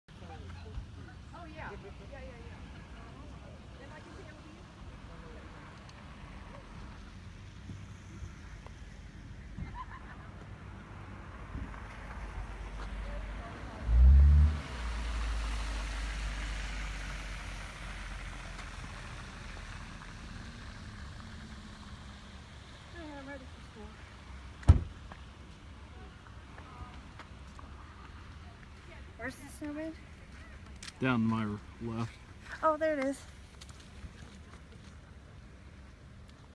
Spring Snow storm
Snowing at New Found Gap in the Great Smoky Mountains.